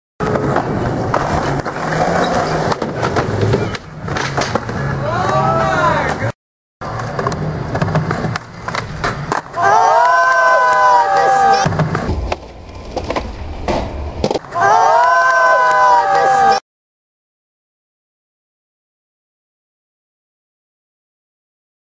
[EPIC] KICK FLIP BS LIP
Hope you liked it,shout out to Tailen for landing this trick
GOD BLESS!